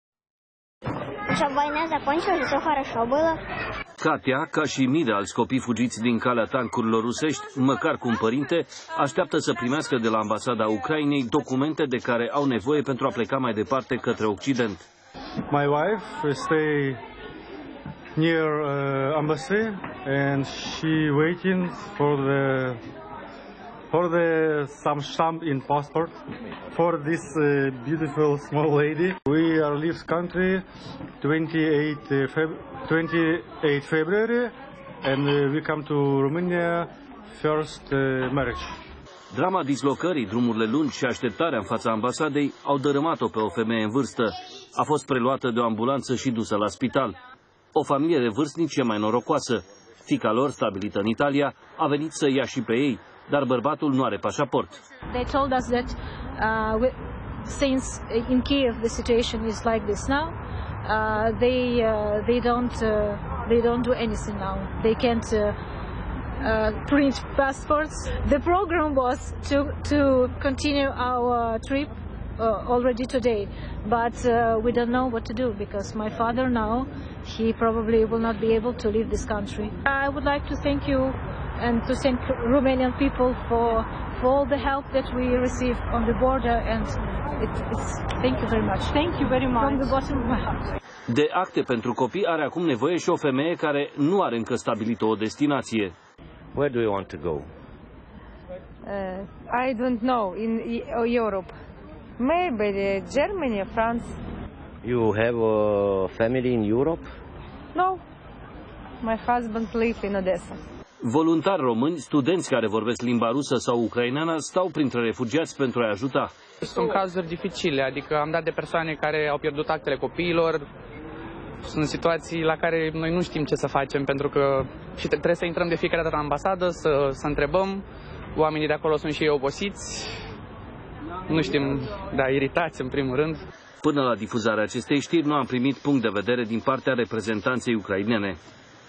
Refugiații fără documente, blocați în România, stau la coadă la Ambasada Ucrainei
Pentru o parte dintre ucrainenii care ajung în România, teama de bombardamnete dispare, dar începe o altă luptă, cea de a obține documente care să le permită să călătorească spre alte state, unde îi așteaptă rude sau prieteni.